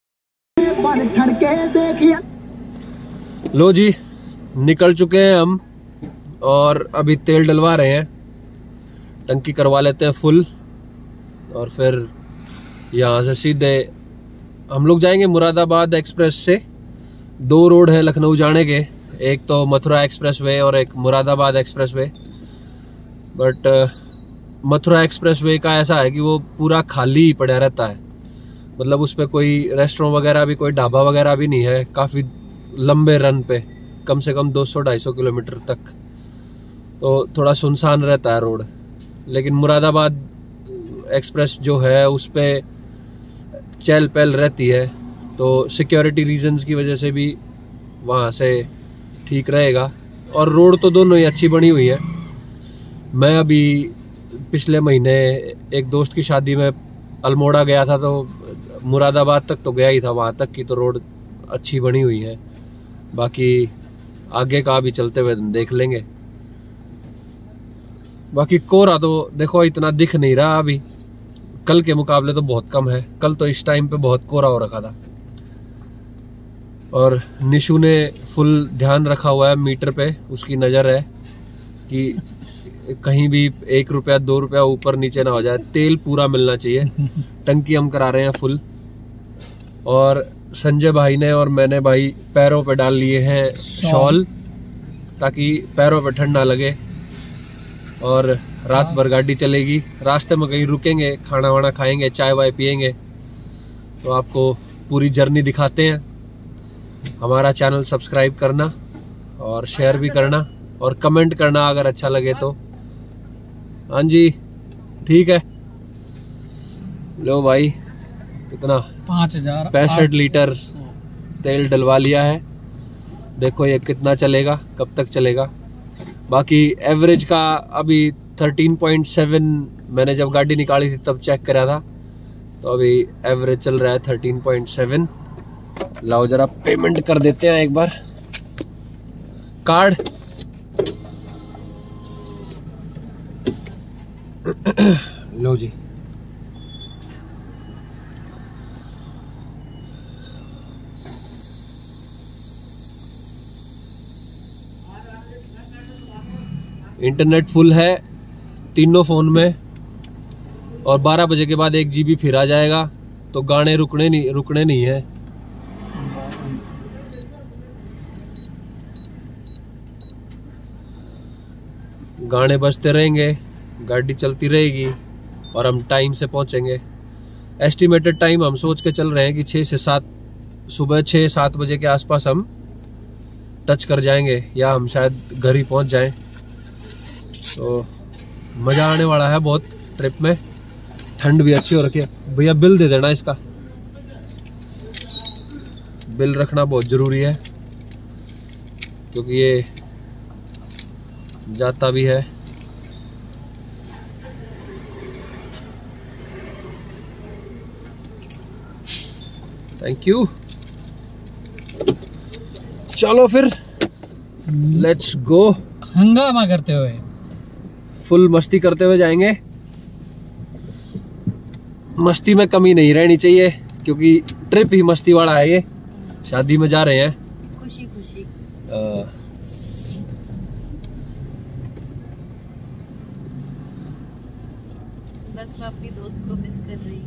lucknow trip - part 1
hum nikle raat me lucknow k liye.
watch us and like our video and sharFurst time hum muslim wedding me ja rahe hai apne friend ki